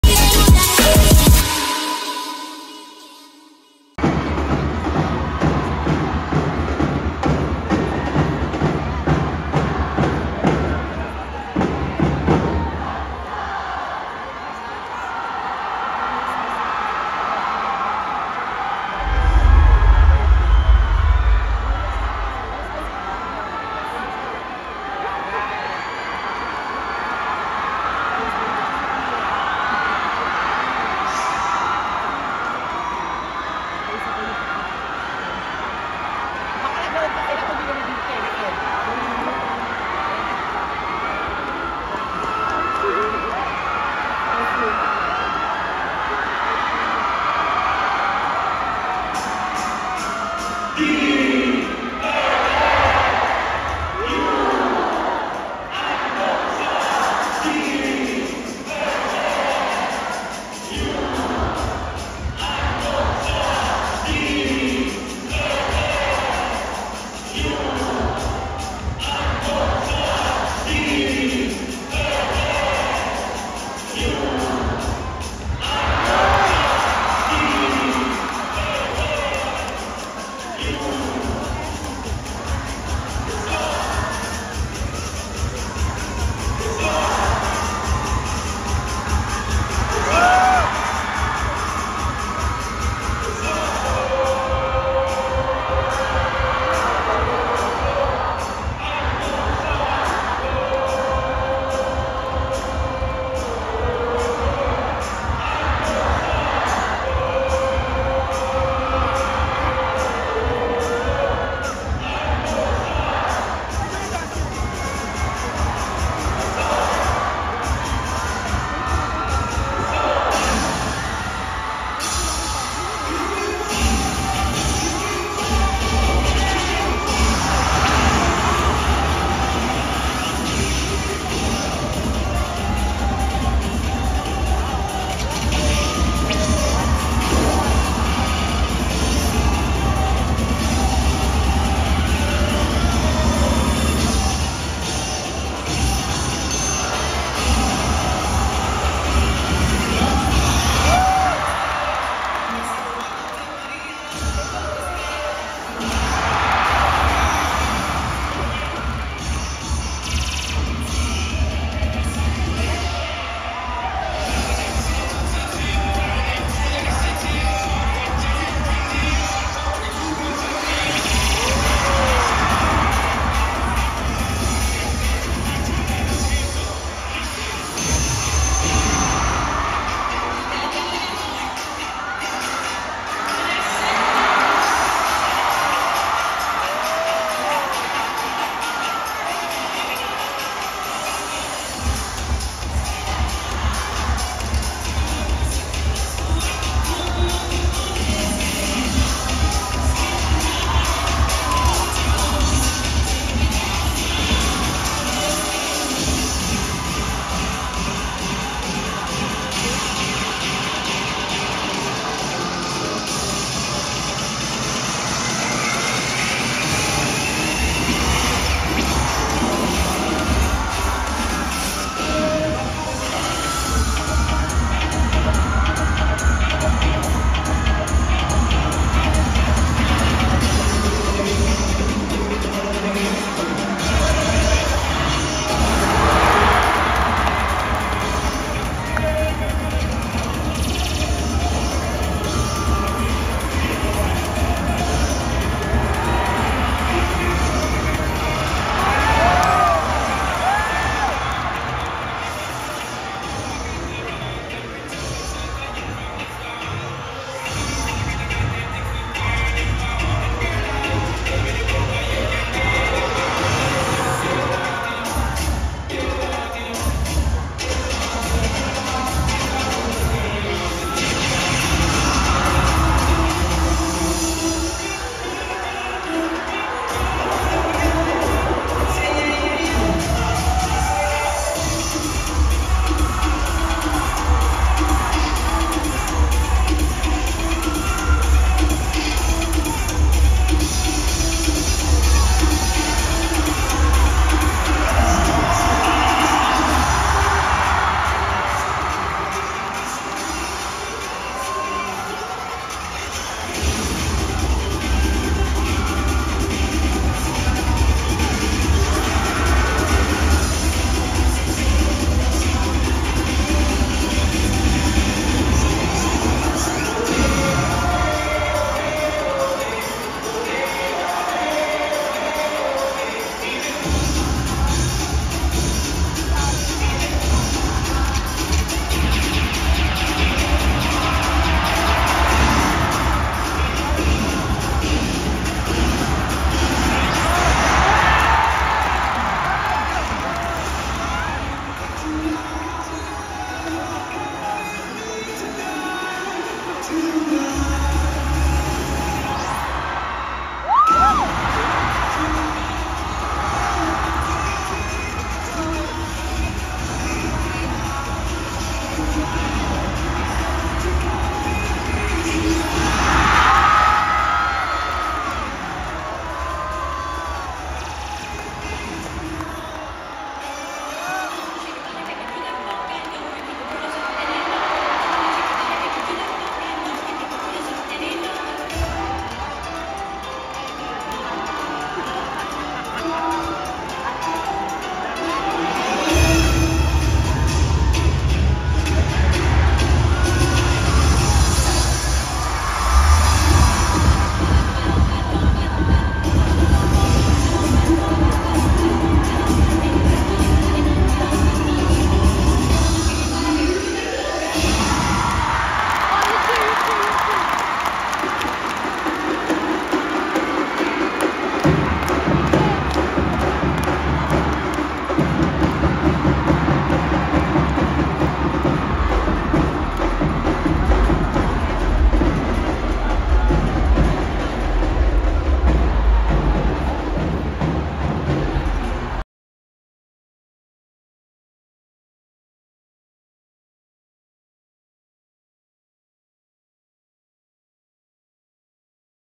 DLSU Animo Squad - Season 86 UAAP Cheerdance Competition
6th Place: DLSU Animo Squad
TUMBLING: 63.5
STUNTS: 62
TOSSES: 62
PYRAMIDS: 52
DANCE: 327
PENALTIES & DEDUCTION: 11
TOTAL SCORE: 555.5

CTTO for Thumbnail Background